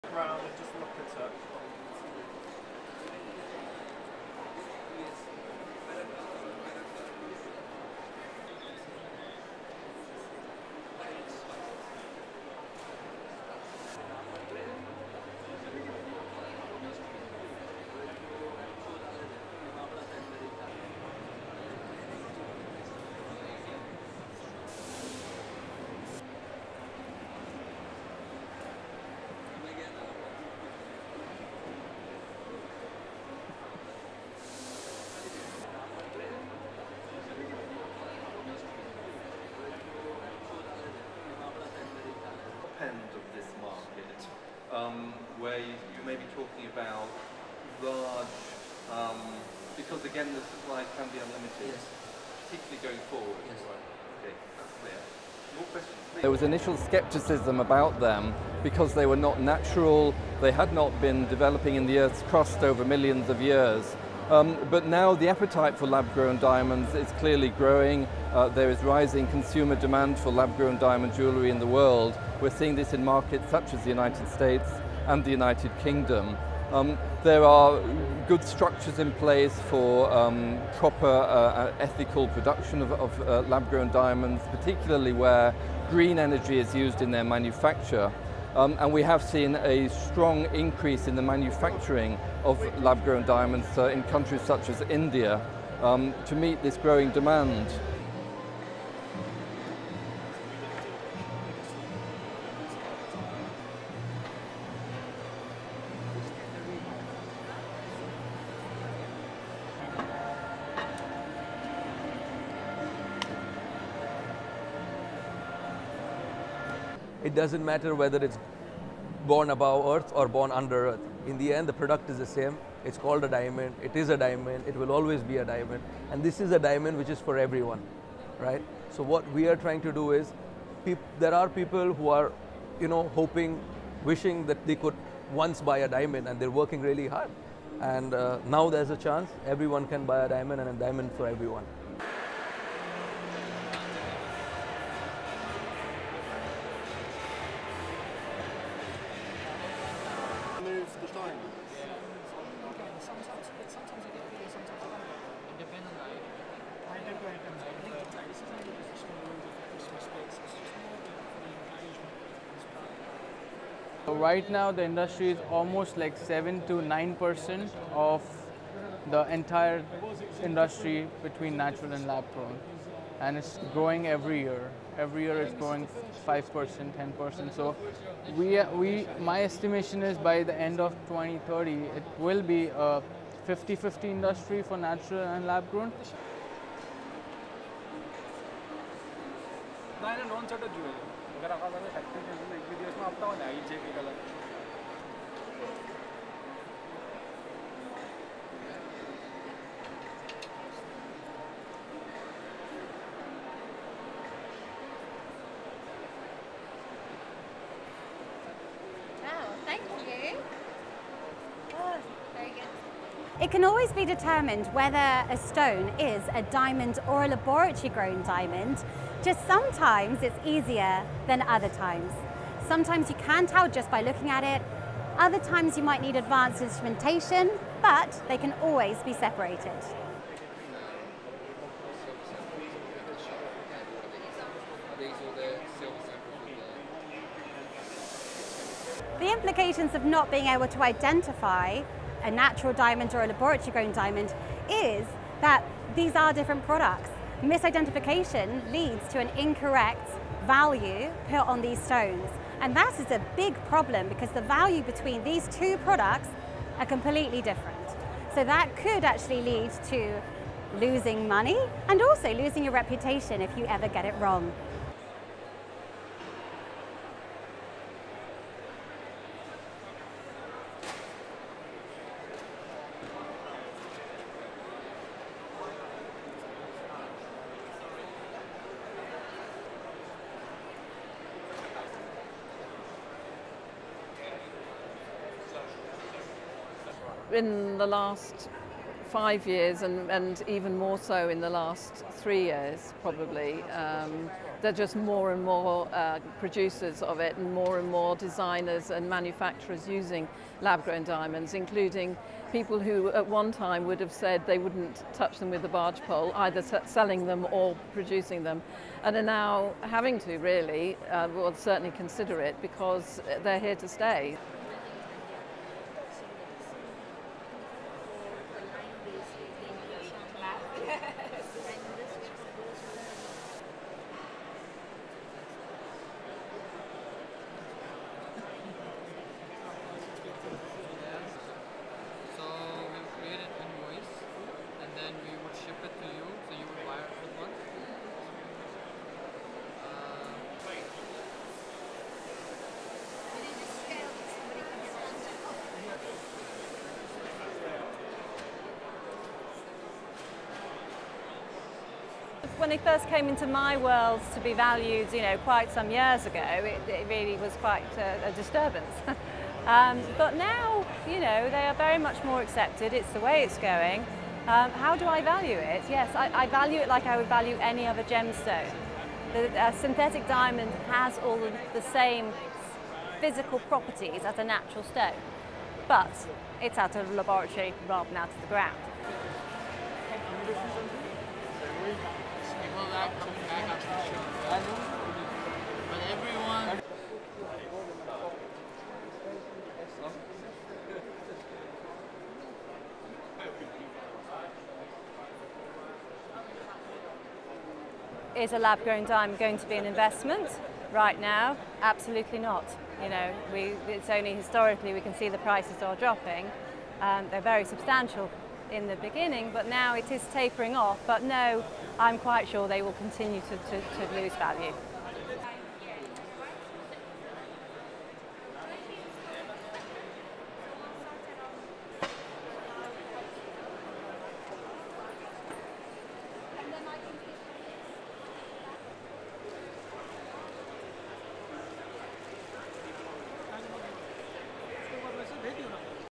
Lab-grown diamonds shine in the marketplace
(7 Sep 2022) UK JEWELLERY

RESTRICTION SUMMARY:
LENGTH: 07:16
ASSOCIATED PRESS
London, UK - 5 September 2022
1. Various of person holding lab-grown diamond with tweezers
2. Close of lab-grown diamond rings on turnstile
3. Various of lab-grown diamond necklace
4. Close of lab-grown diamond earrings on turnstile
5. Various set up shots of David Brough,editor, Jewellery Outlook international jewellery trade magazine, chairing an expert panel talk on lab-grown diamonds
6. SOUNDBITE (English) David Brough, editor, Jewellery Outlook international jewellery trade magazine:
"There was initial scepticism about them because they were not natural. They had not been developing in the Earth's crust over millions of years. But now the appetite for lab-grown diamonds is clearly growing. There is rising consumer demand for lab-grown diamond jewellery in the world. We're seeing this in markets such as the United States and the United Kingdom. There are good structures in place for proper ethical production of lab-grown diamonds, particularly where green energy is used in their manufacture. And we have seen a strong increase in the manufacturing of lab-grown diamonds in countries such as India to meet this growing demand."
7. Set up shot of Sanket Patel, director, Greenlab Diamonds
8. Close of box containing lab-grown diamond
9. Close of lab-grown diamond in the hand, 8.4-carat emerald cut, 2A-quality diamond costing £21,000
10. SOUNDBITE (English) Sanket Patel, director, Greenlab Diamonds:
"It doesn't matter whether it's born above earth or born under earth. In the end the product is the same. It's called a diamond. It is a diamond. It will always be a diamond. And this is a diamond which is for everyone. So what we are trying to do is -- there are people who are hoping, wishing that they could once buy a diamond and they're working really hard. And now there's a chance everyone can buy a diamond and a diamond for everyone."
11. Set up shot of Raj Vaidya
12. Various of tiny lab-grown diamonds being moved with tweezers
13. SOUNDBITE (English) Raj Vaidya, owner, DiamSpark lab-grown diamonds:
"Right now, the industry is almost like 7% to 9% of the entire industry between natural and lab-grown and it's growing every year. Every year it's growing 5%, 10%. So my estimation is by the end of 2030, it will be 50/50 industry for natural and lab-grown."
14. Various of Julia Griffith holding lab-grown diamond up to her loupe
15. SOUNDBITE (English) Julia Griffith, founder, the Gem Academy:
"It can always be determined whether a stone is a diamond or a laboratory-grown diamond. Just sometimes it's easier than other times. Sometimes you can tell just by looking at it. Other times you might need advanced instrumentation, but they can always be separated."
16. Close of lab-grown diamond ring
17. SOUNDBITE (English) Julia Griffith, founder, the Gem Academy:
"The implications of not being able to identify a natural diamond or a laboratory-grown diamond is that these are different products. Misidentification leads to an incorrect value on put these stones, and that is a big problem because the value between these two products are completely different. So that could actually lead to losing money and also losing your reputation if you ever get it wrong."
18. Mid of sign reading: (English) "Diamonds for everyone"
19. Various set up shots of Belinda Morris, editor, The Jeweller
20. SOUNDBITE (English) Belinda Morris, editor, The Jeweller:
21. Mid zoom of boxes of lab-grown diamonds with different cuts
22. Various of various lab-grown diamonds
28. Various of the Jewellery Show